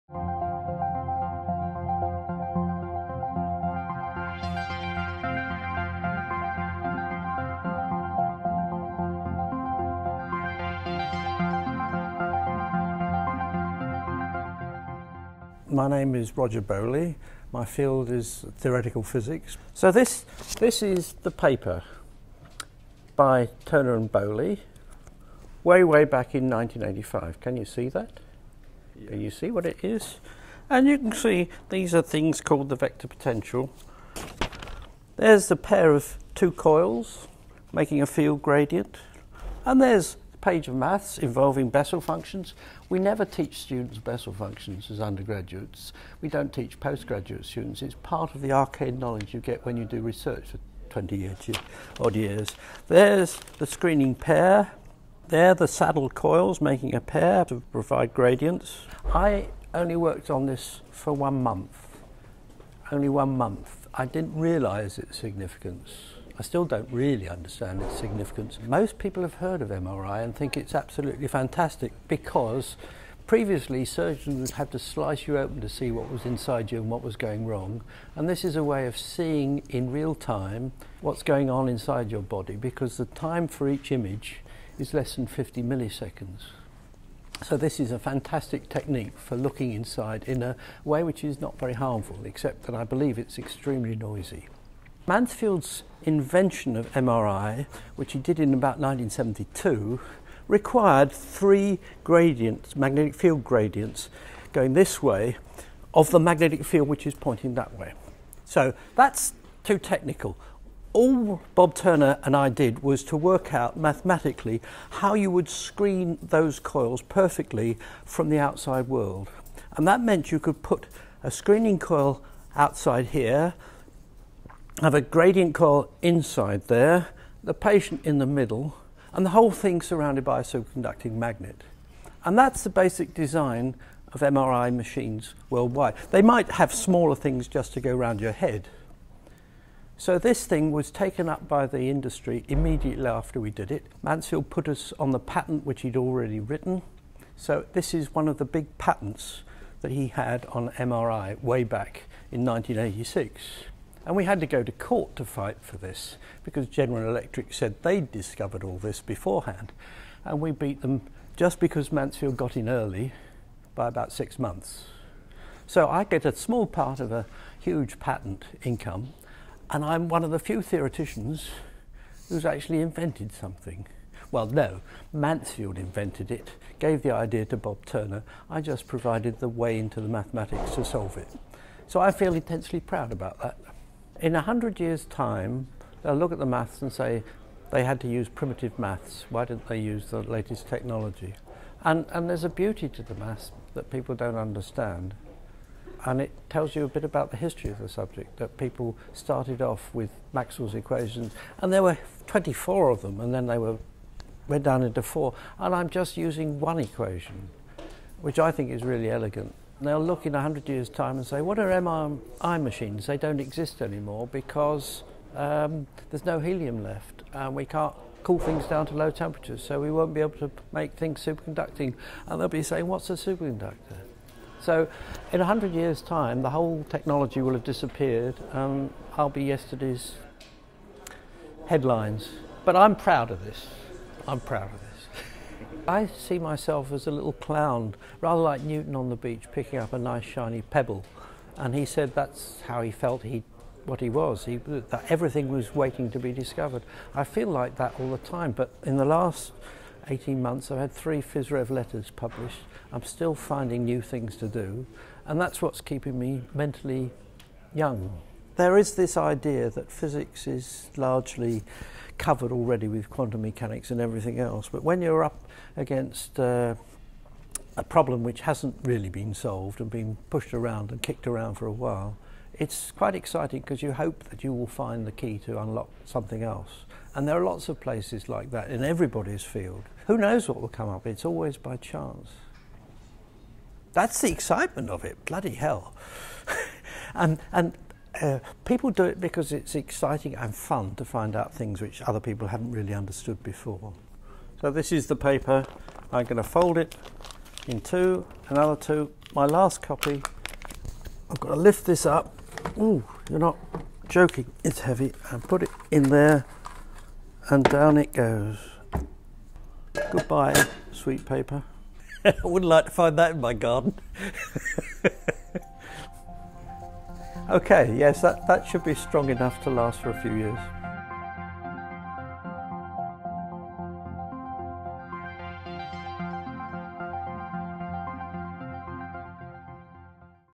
Time Capsule #3 - Roger's Paper
Professor Roger Bowley explains why he is putting an academic paper in the time capsule.

This film was shot and edited by James Hennessy.

Test Tube is a project by video journalist Brady Haran for the University of Nottingham.